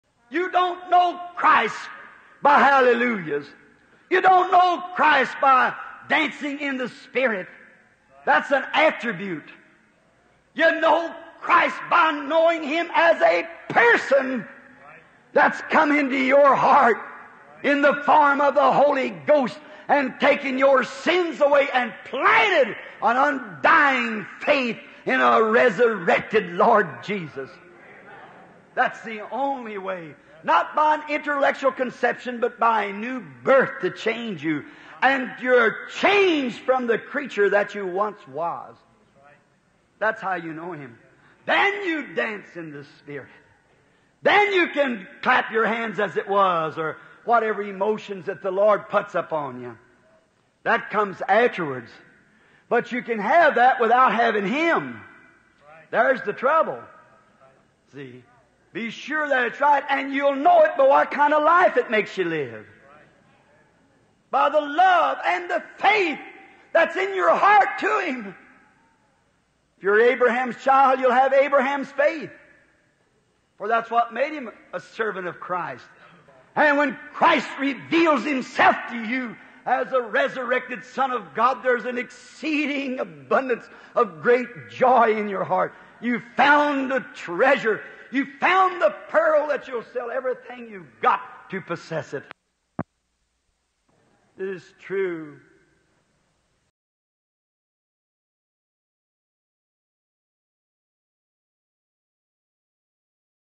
How to know Christ | William Branham
IT IS TRUE
You don’t know Christ by hallelujahs. You don’t know Christ by dancing in the Spirit; that’s an attribute. You know Christ by knowing Him as a Person that’s come into your heart in the form of the Holy Ghost, and taken your sins away, and planted an undying faith in a resurrected Lord Jesus. That’s the only way. Not by an intellectual conception, but by a new birth to change you. And you’re changed from the creature that you once was. That’s how you know Him. Then you dance in the Spirit. Then you can clap your hands, as it was, or whatever emotions that the Lord puts upon you. That comes afterwards. But you can have that without having Him. There’s the trouble. See? Be sure that it’s right. And you’ll know it by what kind of life it makes you live, by the love and the faith that’s in your heart to Him. If you’re Abraham’s child, you’ll have Abraham’s faith, for that’s what made him a servant of Christ.
   58-0112A - His Wonders To Perform
   Rev. William Marrion Branham